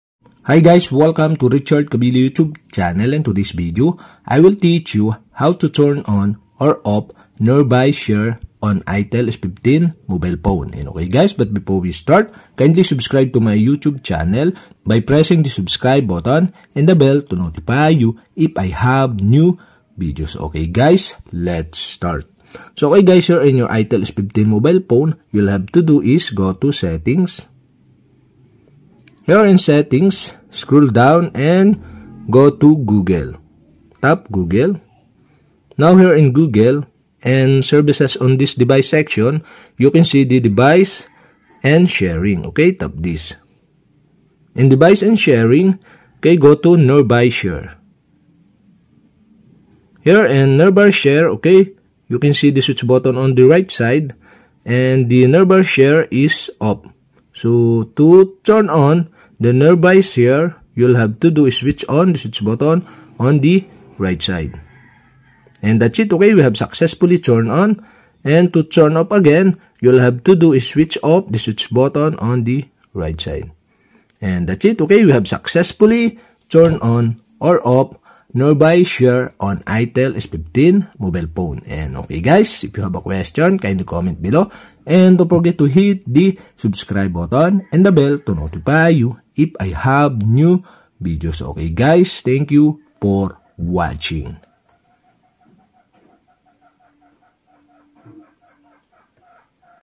How to turn on or off nearby share on itel S15 | Device & Sharing | Google
Do you want to turn on or off nearby share on itel S15 mobile phone.

Thanks...